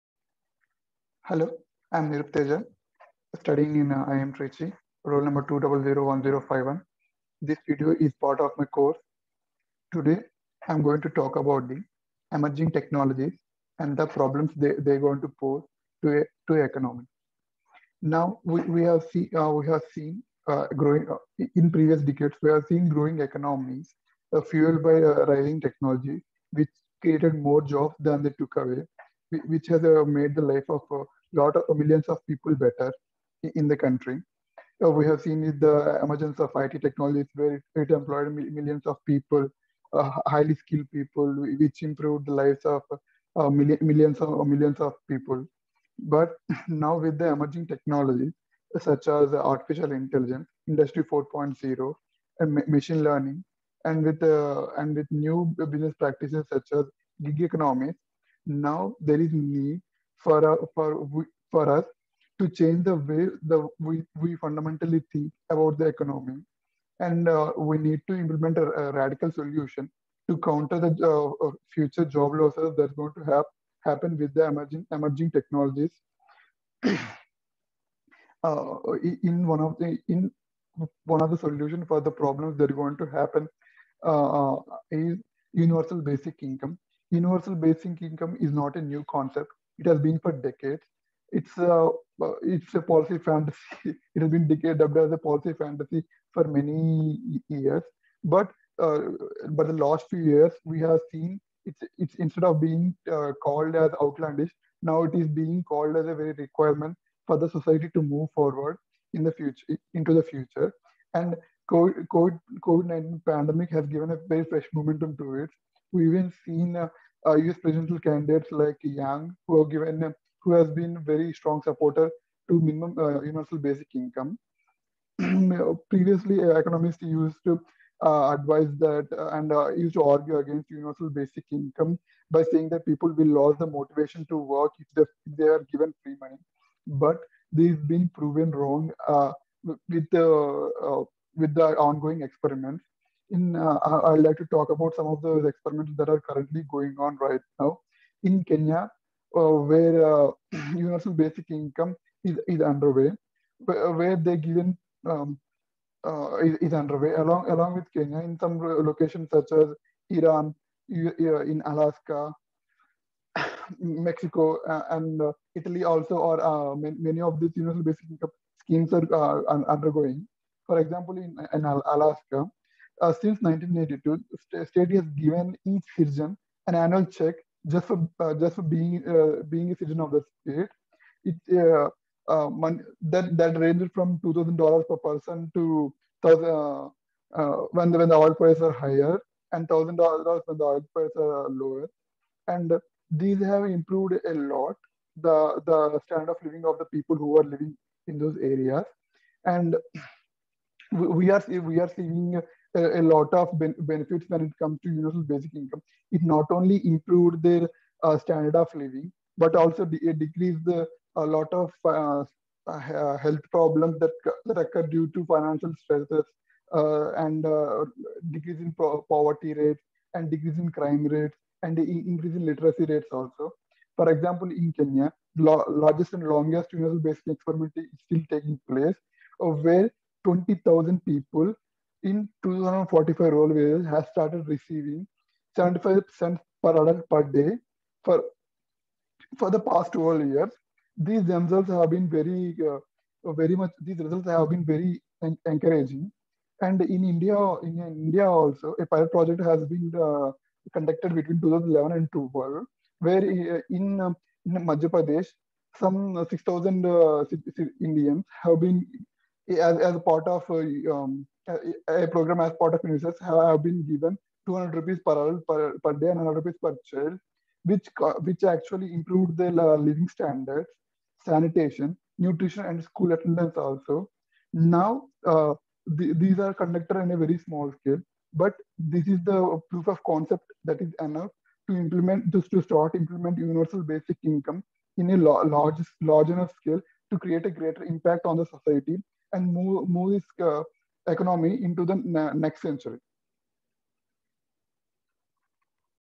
Necessity of Universal Basic Income | Niroop Teja | 2001051
In this video I talk about the need for immediate necessity for the implementation of Universal Basic Income throughout the country